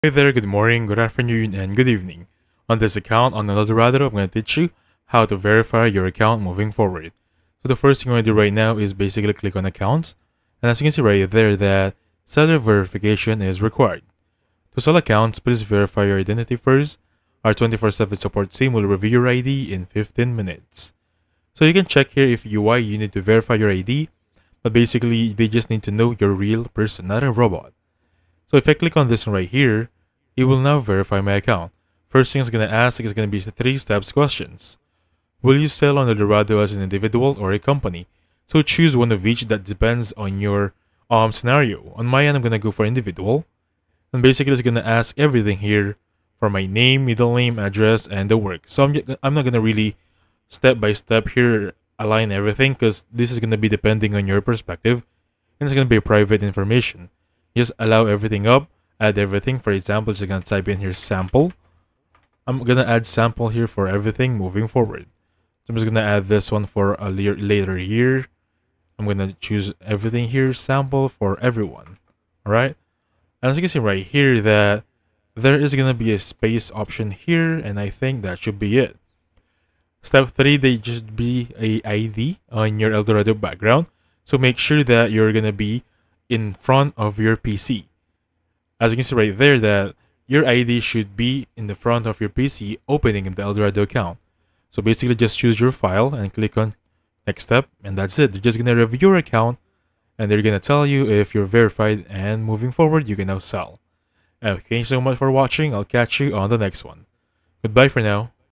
Eldorado.gg Account Verification Made Easy – Complete Guide 2025 📲✅🔔
Eldorado.gg Account Verification Made Easy (2025) | Step-by-Step Guide ✅📲
Need help verifying your Eldorado.gg account in 2025? This quick and easy tutorial walks you through the entire account verification process, including ID upload, phone confirmation, and tips to get verified fast – so you can start buying or selling in no time!

🛒 In This Video, You’ll Learn:
✔️ How to verify your Eldorado.gg account (2025 update)
✔️ What documents are required
✔️ How to fix common verification issues
✔️ Tips for faster approval
✔️ Safe and legit steps for new users

📌 Verification is required to access full features and ensure account security.